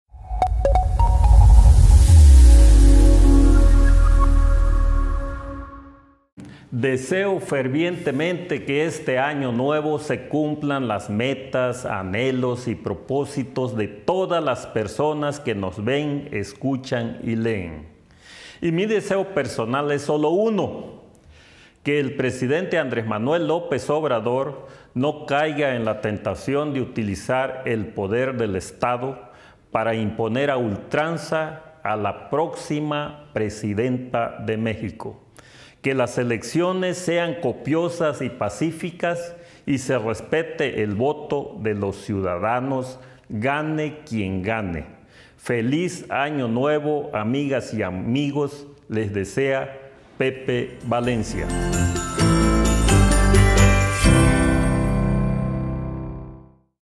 ADELANTE- Deseo único para el 2024
Pepe Valencia nos da su opinión: Que AMLO no intente hacer fraude o imponer por la fuerza a Claudia Sheinbaum